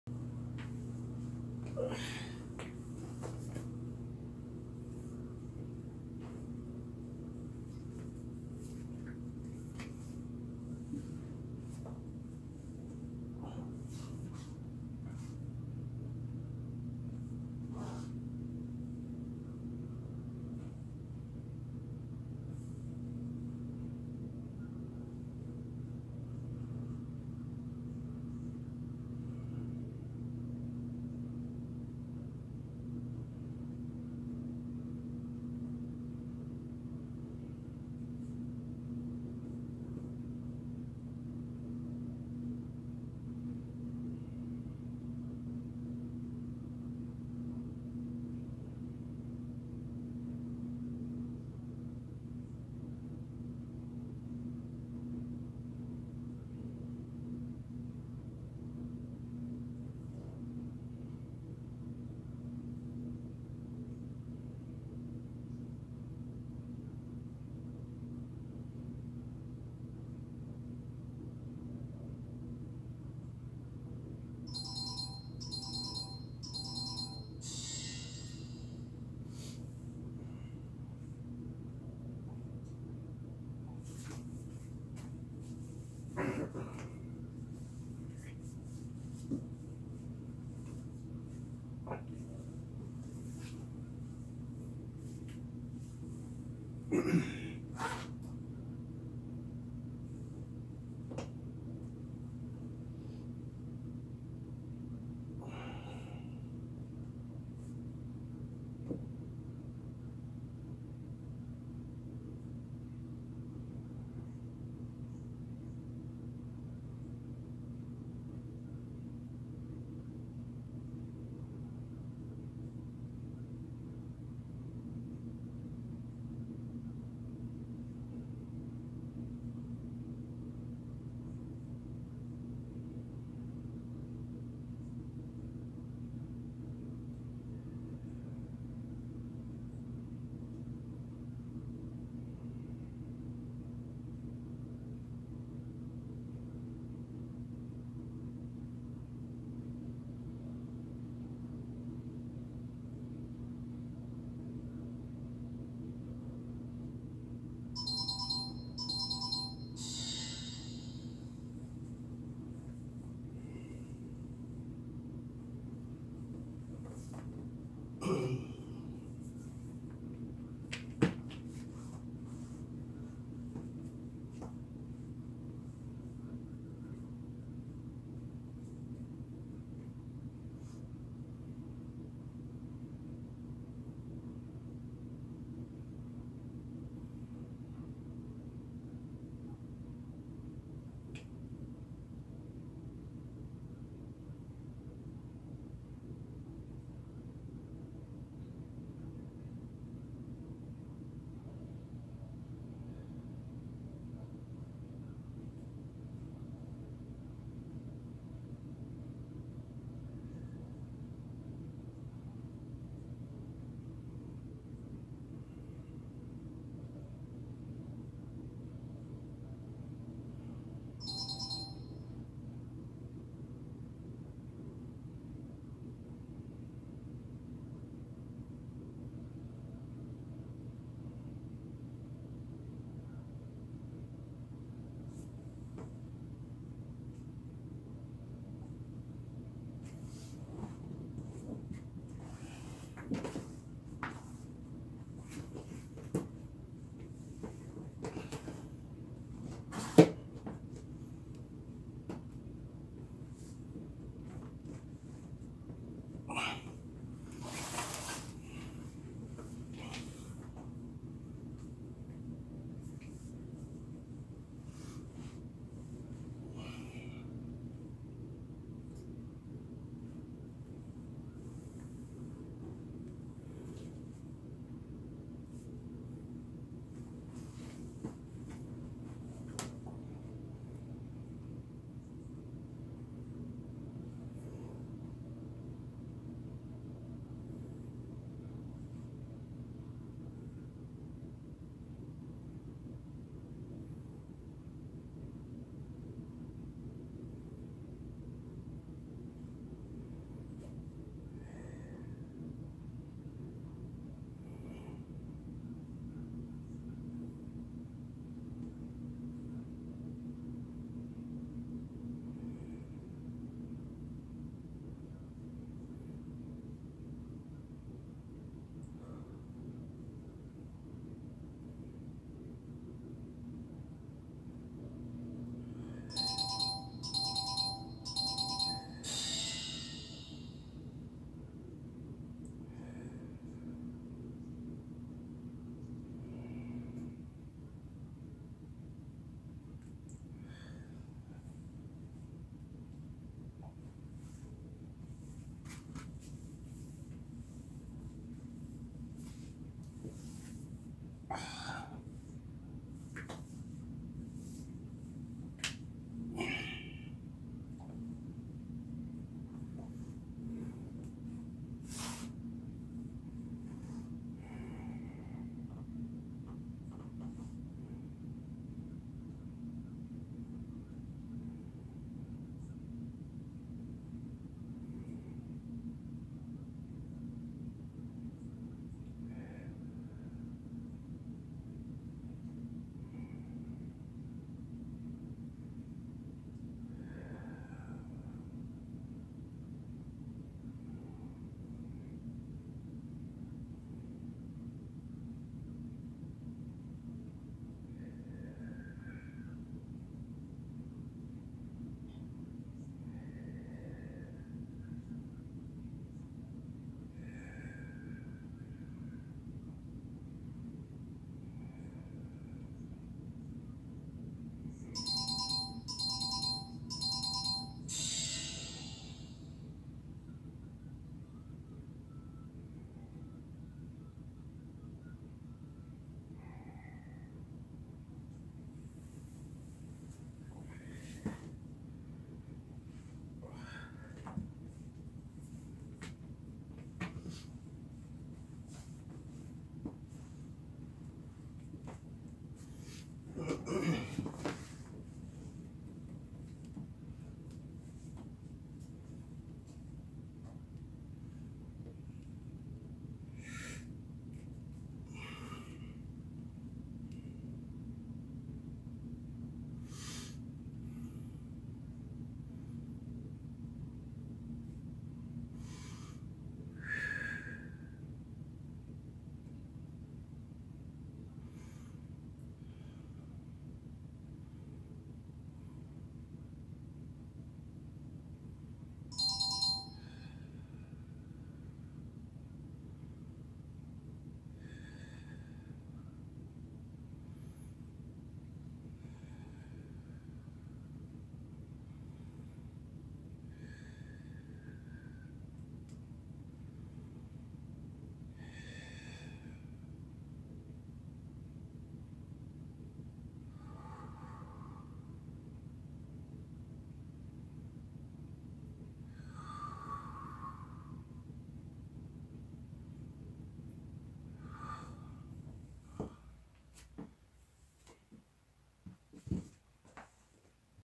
Étirement Quadriceps vaste externe partie latéral muscle Quadriceps
Étirement Quadriceps portion latéral ( vaste externe) garde les genoux collés . Flexion dorsal du pied en tout temps. Bassin en retroversion. Colle le nombril au sol pendant une minute. Auto agrandir tout le corps .

3 portions ici sont à étirer:
Vaste médial, vaste latéral et droit antérieur